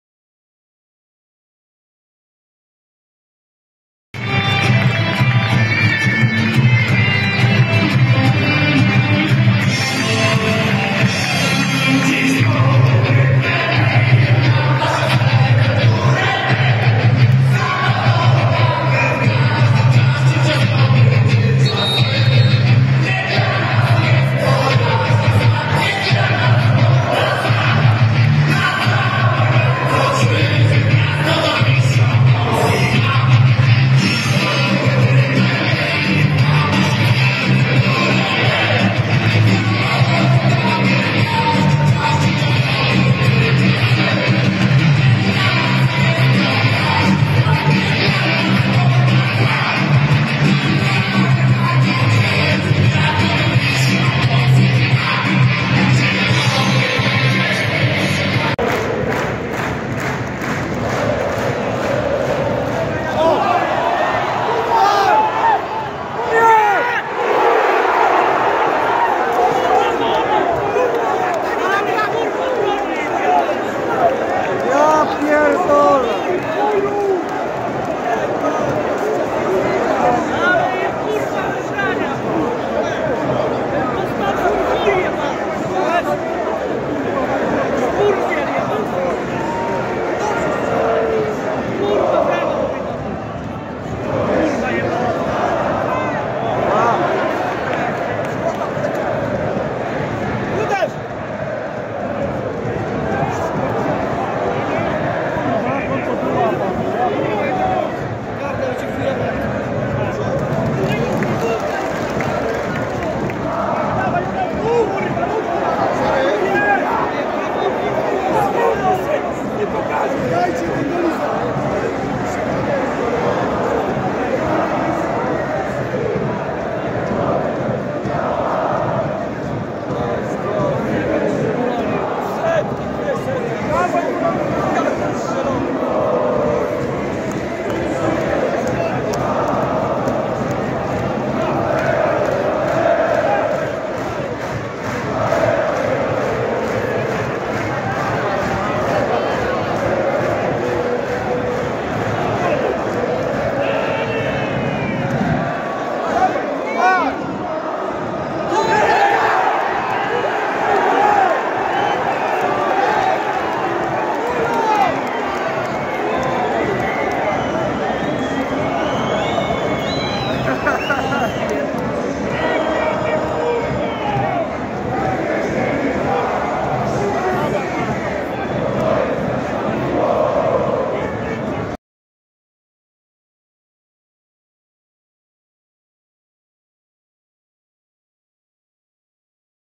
Kibice Motoru Lublin na meczu z Górnikiem Łęczna. Baraże o Ekstraklasę | 30.05.2024
► Baraże o Ekstraklasę - półfinały: Motor Lublin 0-0 pd. k. 4-2 Górnik Łęczna, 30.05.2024